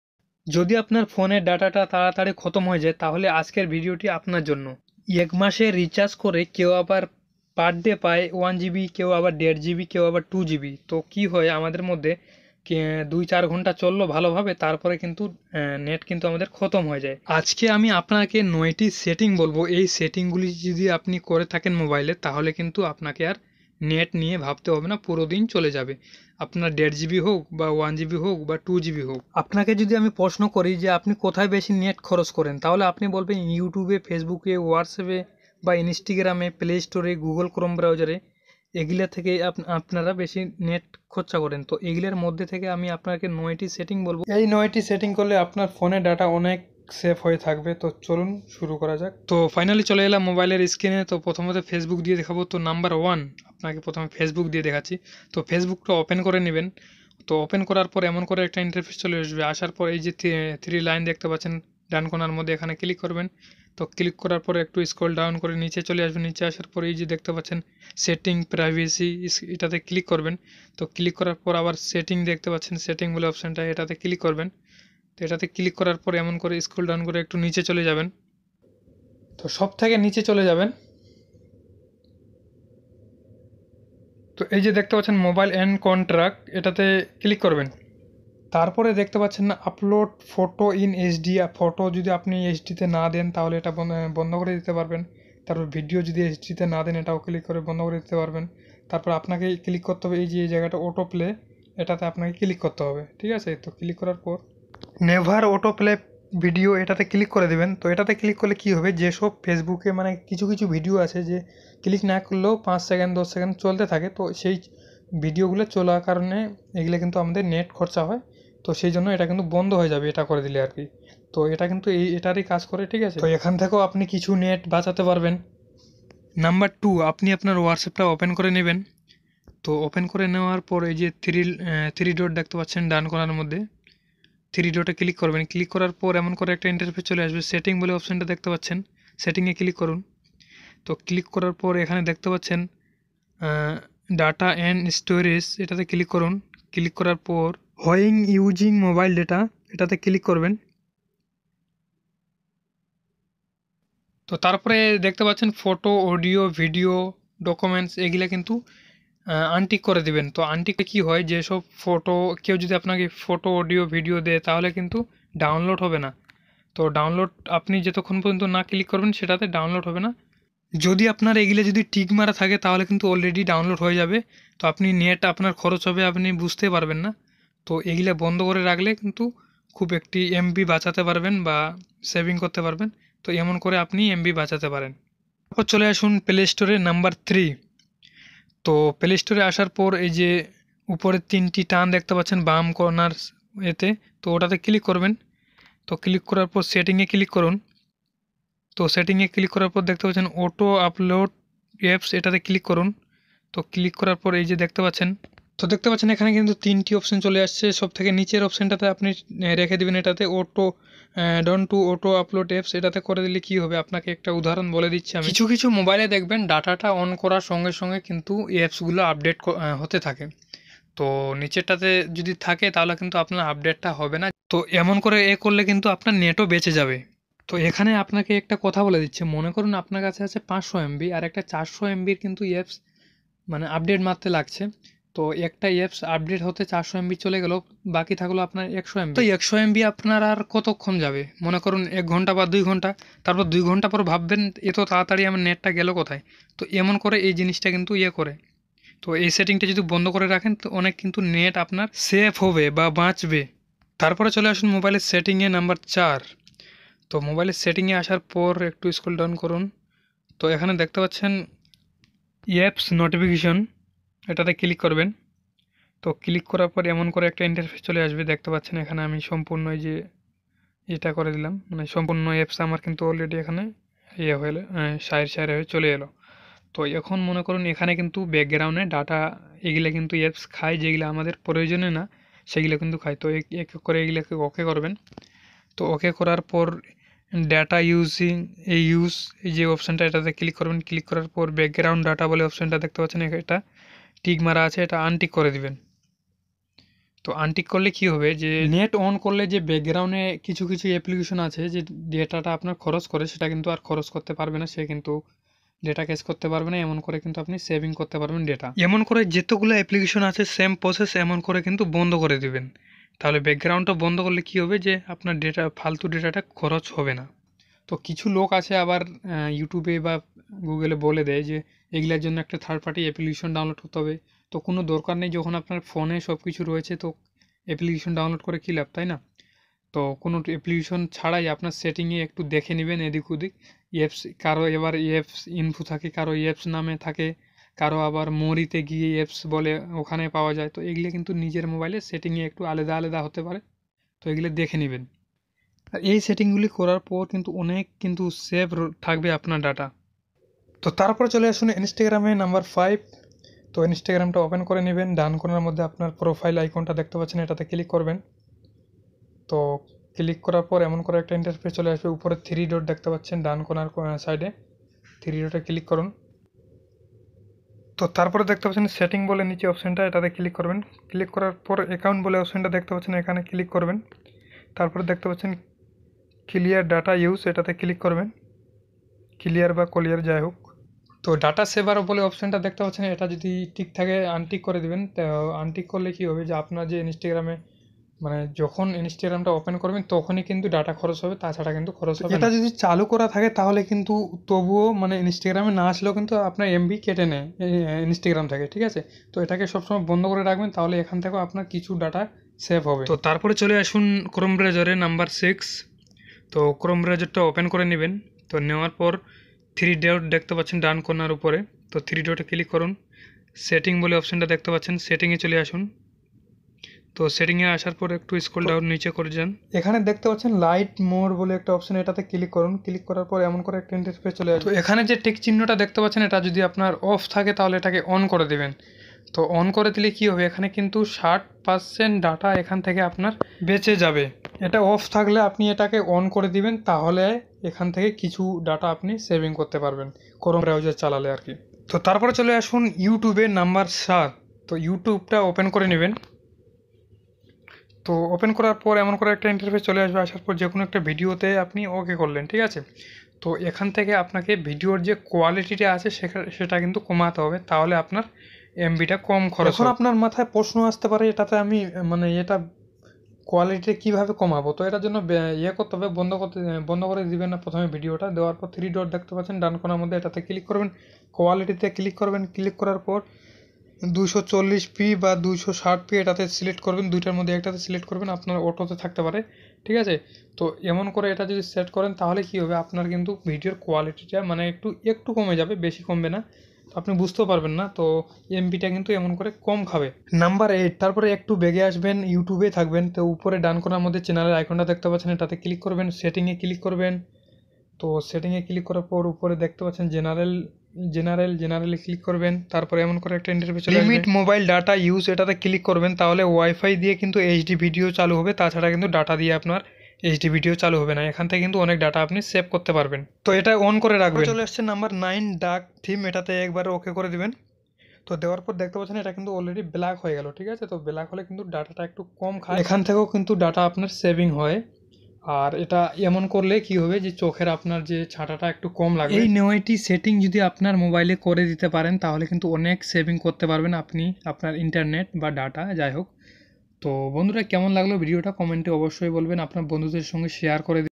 mobile Data saving Tips মোবাইলের অতিরিক্তMB কাটা বন্ধ হবে
Hello friends how are you all hope you are all well

After doing these settings today, your phone's internet data will be very less